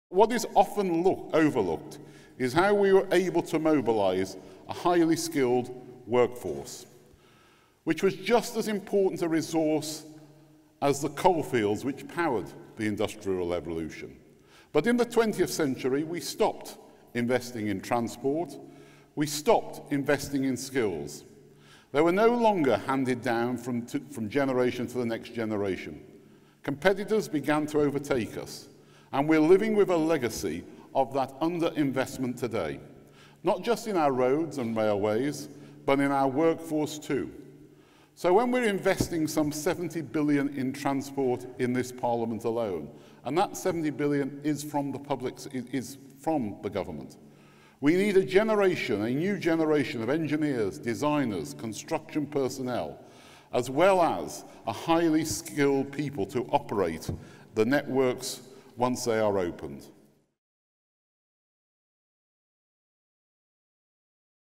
Rt Hon Patrick McLoughlin MP, Secretary of State for Transport - Construction News Summit 2015
summit.cnplus.co.uk